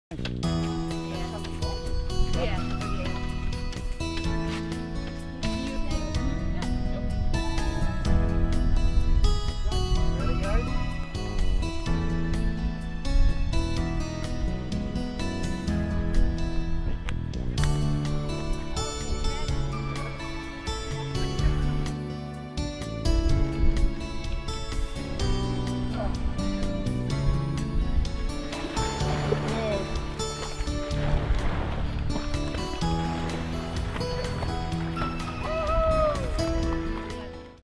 Sea turtle returns to the ocean off the Florida Keys | 10 Tampa Bay
Maisy the hawksbill-green sea turtle returned to the ocean off the Florida Keys Friday. Maisy received a satellite tracking transmitter so she could participate in Tour de Turtles, a "race" that tracks sea turtle migration. The event raises awareness for sea turtle survival.